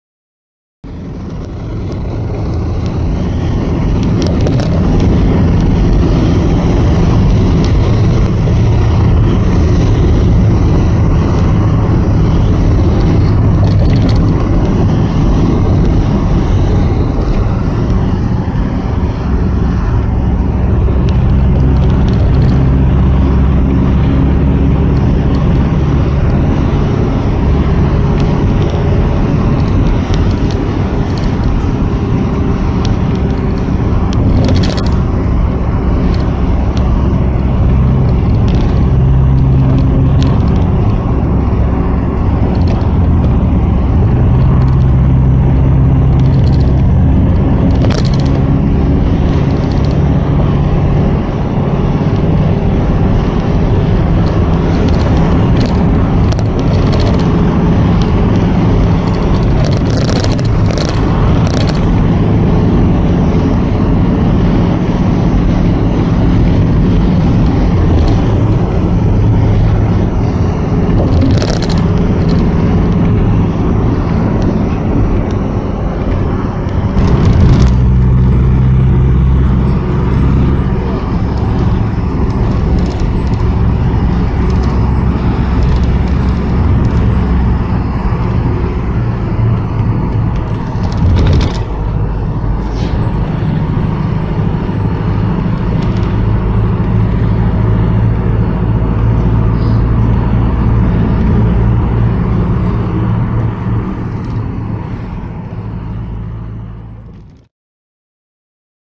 On The Dongseo Elevated Highway Heading East And Looking South In Busan
I recorded this video on my LG F200-L Vu+ Optimus Vu II at 720p as I was in a bus travelling west on the Dongseo Elevated Highway and looking south with the phone pressed directly against the slightly tinted window. The recording started near the Nakdong Bridge.

Date of recording: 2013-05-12 at approximately 19:30.